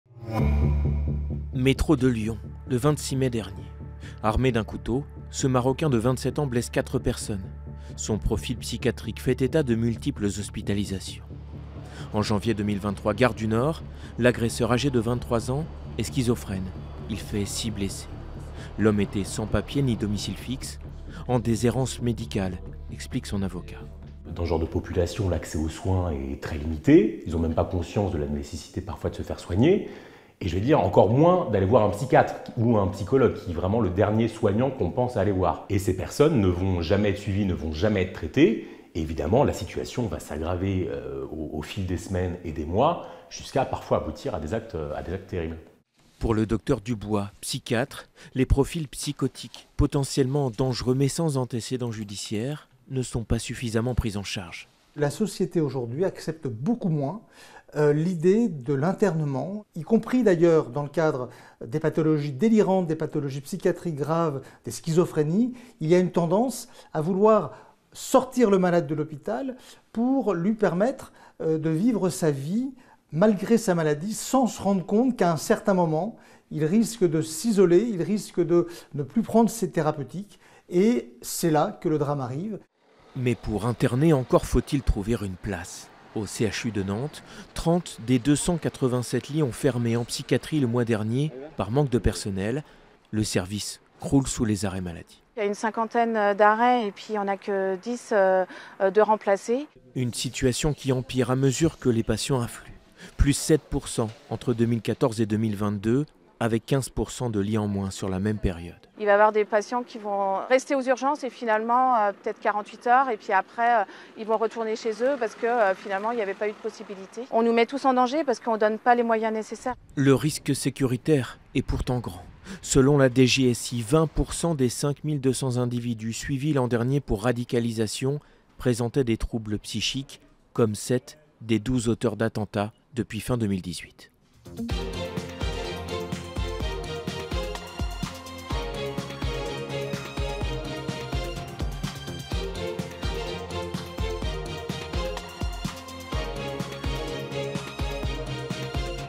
Psychiatrie : le difficile suivi des auteurs de violences
Les auteurs d’attentats ou de violences présentent souvent des troubles psychiques. Après le passage à l’acte, ils font l’objet d’un suivi psychiatrique qui connaît parfois des ratés.